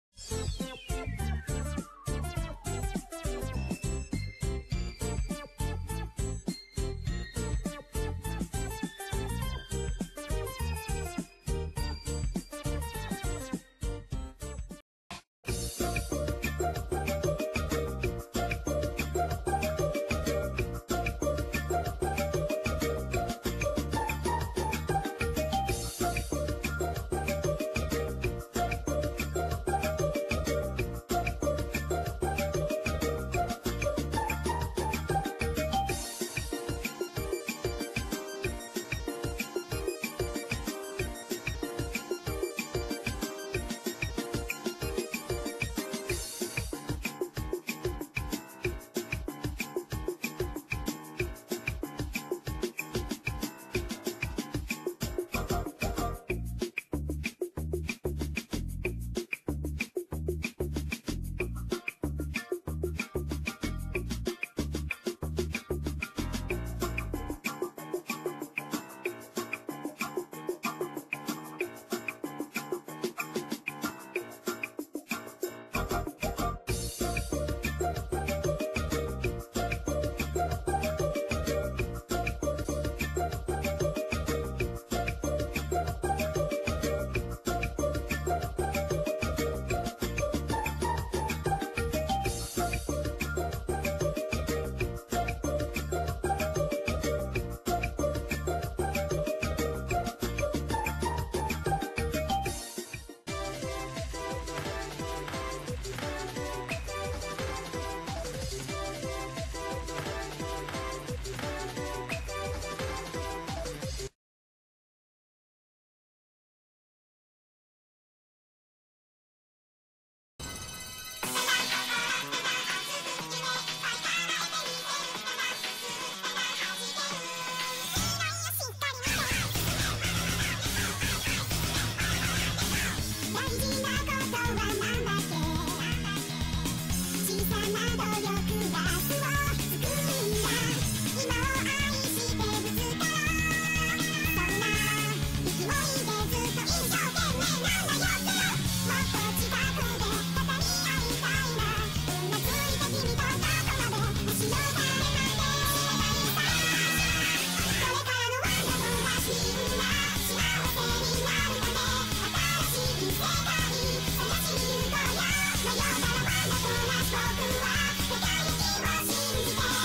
[Old] Gakuen no Yuutsu [Fanime Ep. 1]
An episode of an old fanime I made in 2012! Finally reuploaded it. Forgive the shota comment I was a kid.